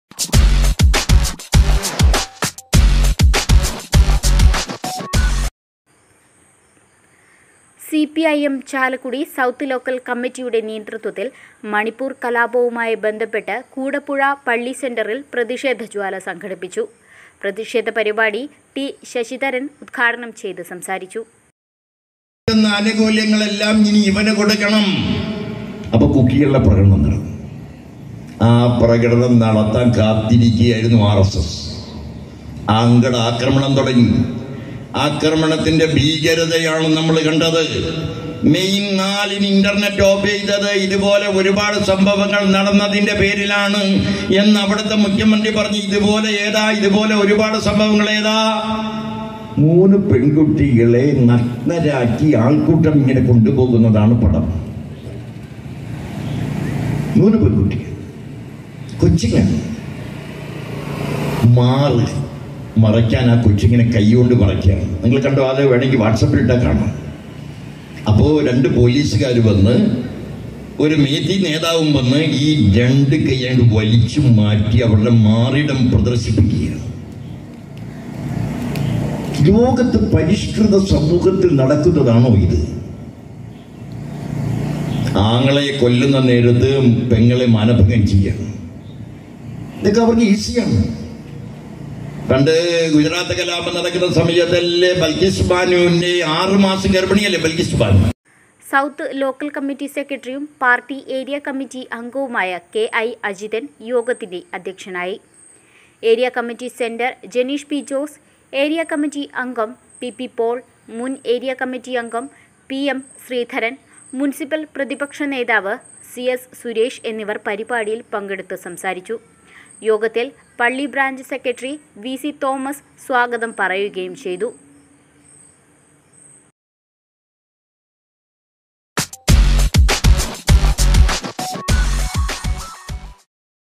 മണിപ്പൂർ കലാപവുമായി ബന്ധപ്പെട്ട് കൂടപ്പുഴ പള്ളി സെന്ററിൽ പ്രതിഷേധ ജ്വാല
CPI (M) ചാലക്കുടി സൗത്ത് ലോക്കൽ കമ്മറ്റിയുടെ നേതൃത്വത്തിൽ മണിപ്പൂർ കലാപവുമായി ബന്ധപ്പെട്ട് കൂടപ്പുഴ പള്ളി സെന്ററിൽ പ്രതിഷേധ ജ്വാല സംഘടിപ്പിച്ചു. പ്രതിഷേധ പരിപാടി സT ശശിധരൻ ഉദ്ഘാടനം ചെയ്ത് സംസാരിച്ചു.സൗത്ത് LC സെ ക്രട്ടറിയും പാർട്ടി AC അംഗവുമായ സ KI അജിതൻ യോഗത്തിന്റെ അദ്ധ്യക്ഷനായി. AC സെന്റർ സ ജെനീഷ് പി ജോസ് AC അംഗം സ PP പോൾ, മുൻ AC യംഗം സ PM ശ്രീധരൻ , മുൻസിപ്പൽ പ്രതിപക്ഷ നേതാവ് സ CS സുരേഷ് എന്നിവർ പരിപാടിയിൽ പങ്കെടുത്തു സംസാരിച്ചു. യോഗത്തിൽ പള്ളി ബ്രാഞ്ച് സെക്രട്ടറി സ VC തോമസ് സ്വാഗതം പറയുകയും ചെയ്തു.